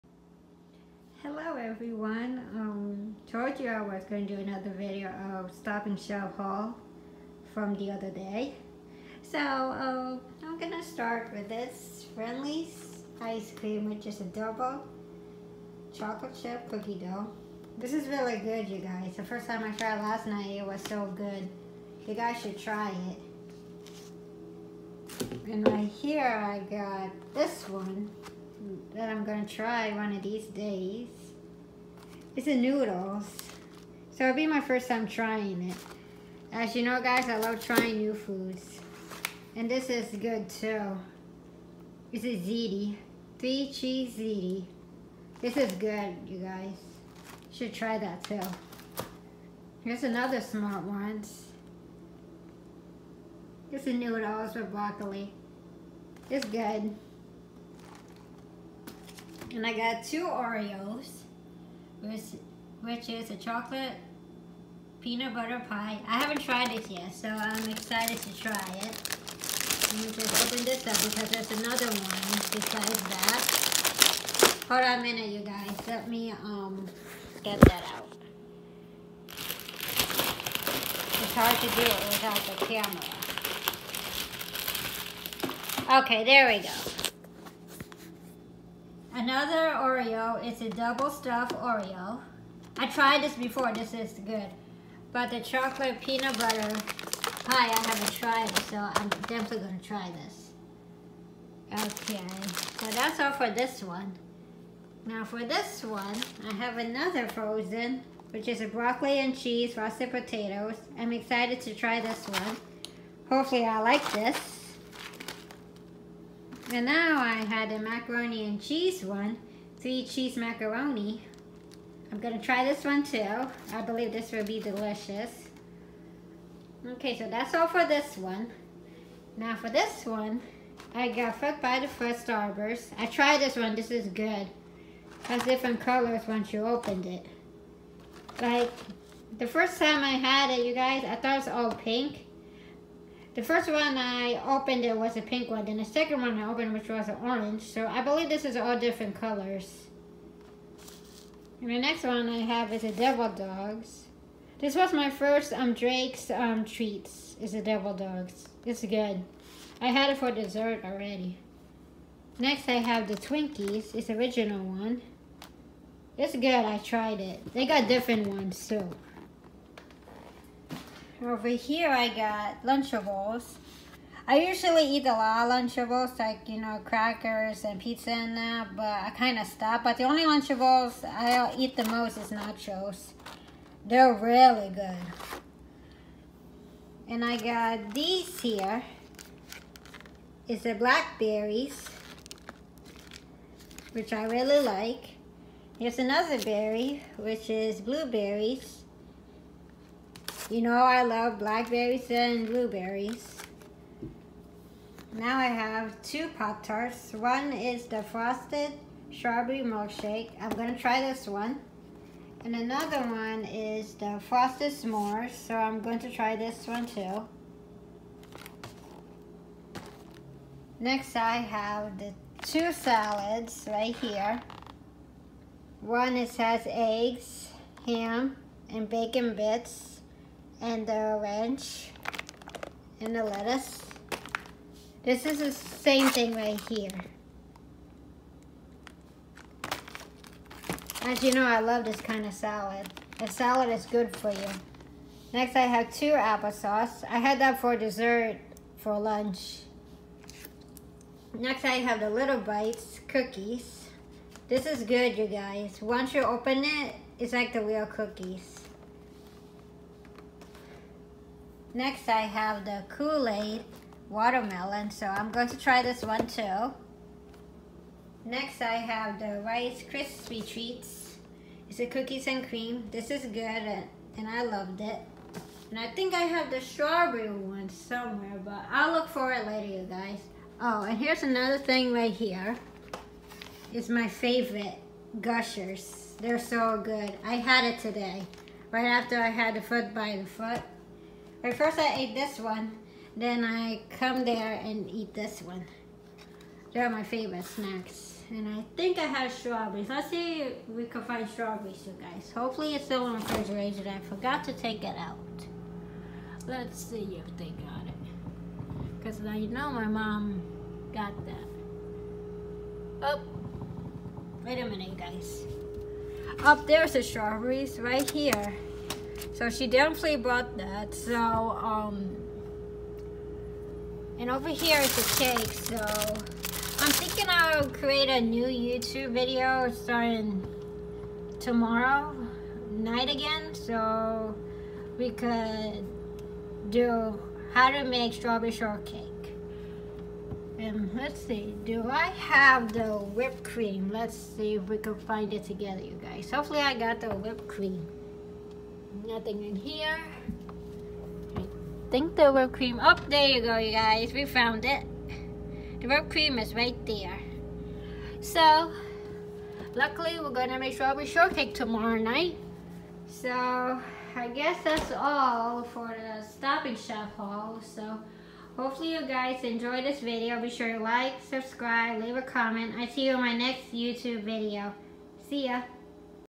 Stop N Shop Haul
Hope you guys enjoy this video lucky I got snacks don’t forget to like and leave a comment love you guys